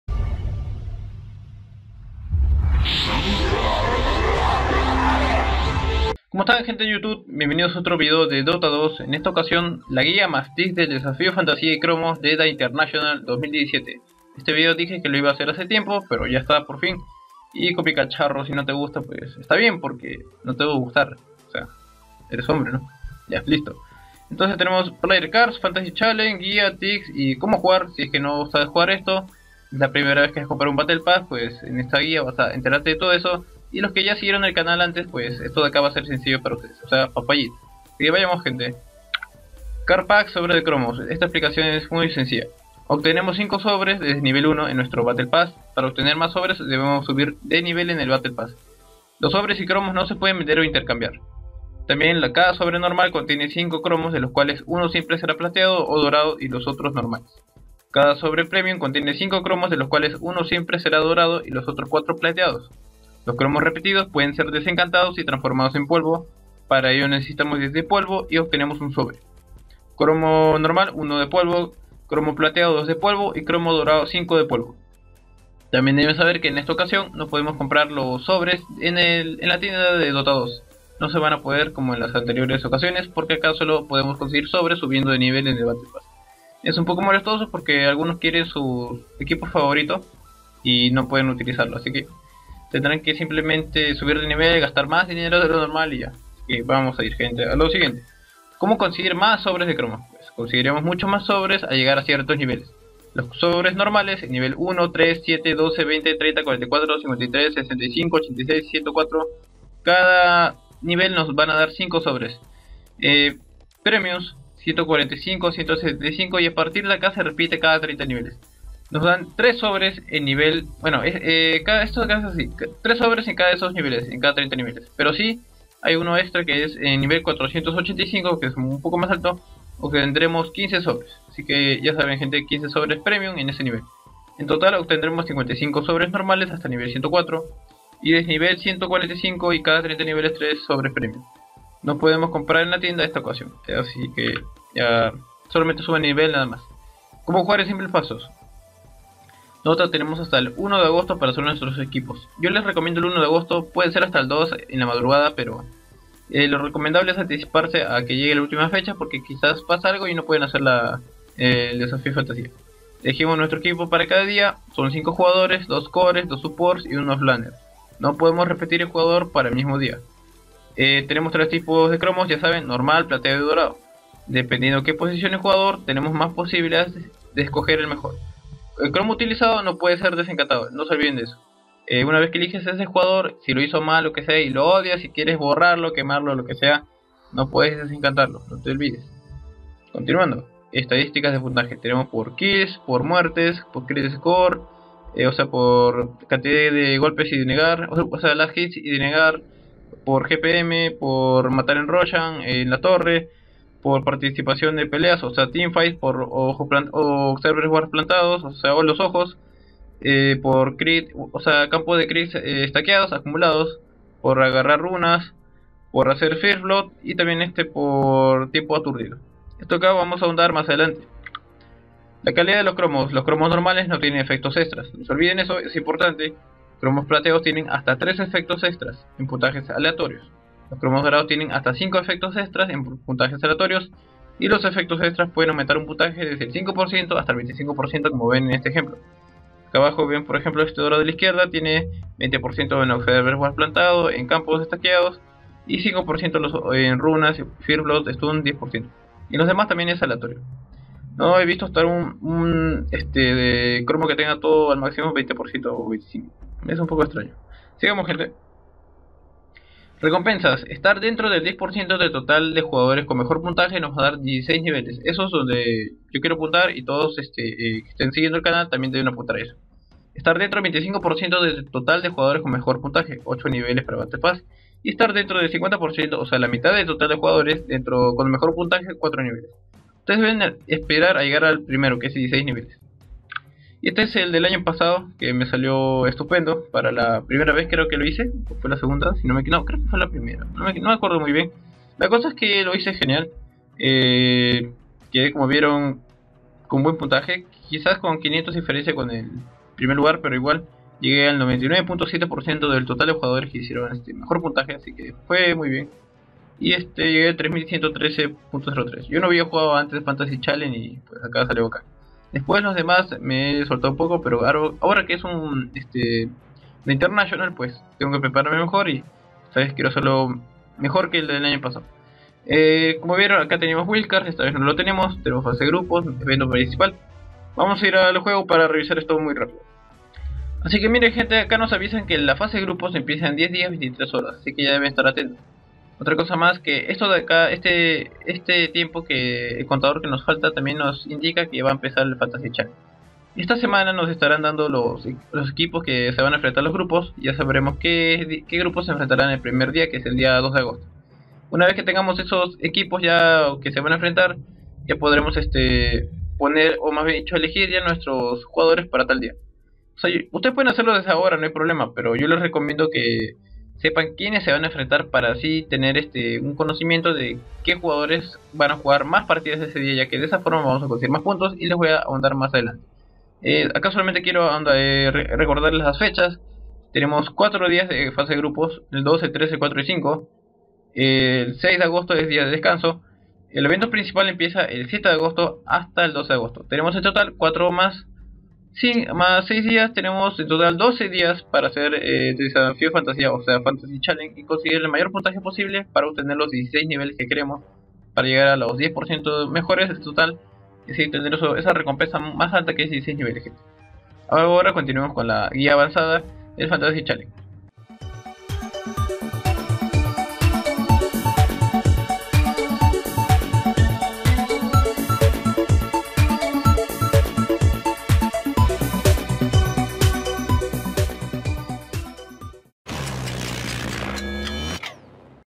BP TI 2017 - ¿Qué es y Cómo jugar el Fantasy Challenge?
El vídeo para saber de qué trata el Fantasy Challenge, si nos sirve para algo y cómo jugarlo está aquí. En la parte 2 están los consejos avanzados de la guía.

Todas las imágenes y vídeos pertenecen a sus respectivos autores. La intención de estos vídeos es promover Dota 2 informándoles a la comunidad de habla española en el juego.